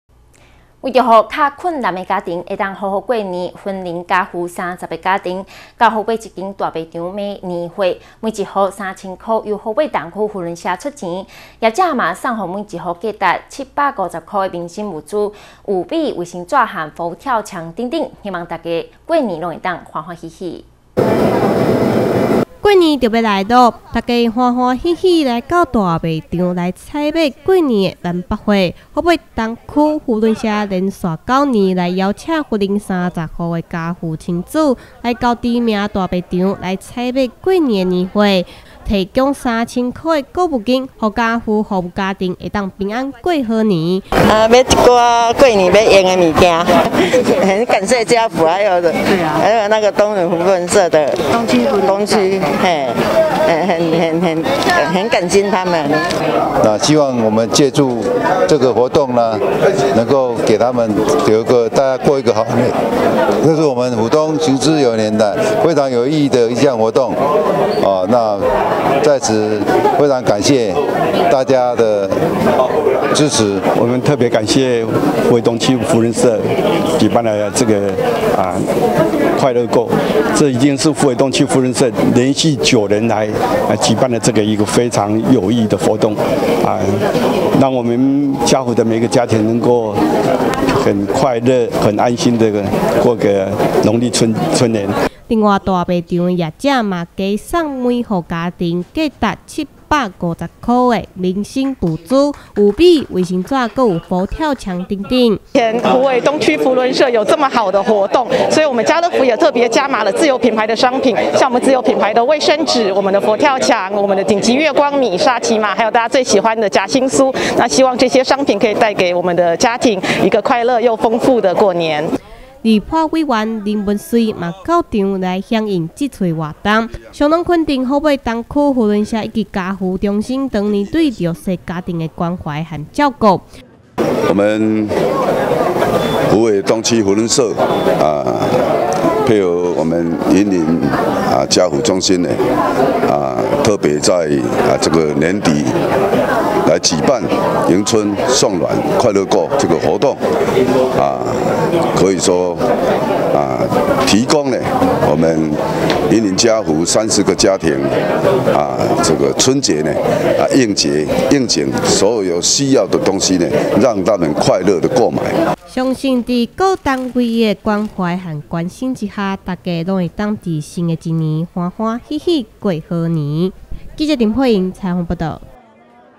虎尾扶輪社寒冬送暖！ 雲林家扶親子開心採買年貨-雲林新聞網
過年迎新春，雲林家扶30戶親子家庭到虎尾家樂福買年貨，三千元購物金由虎尾東區扶輪社出資，虎尾家樂福也加碼送給每戶價值七百五十元的民生物資，有米、衛生紙還有佛跳牆等等，希望大家可以在新的一年歡歡喜喜讓大家能開心迎兔年。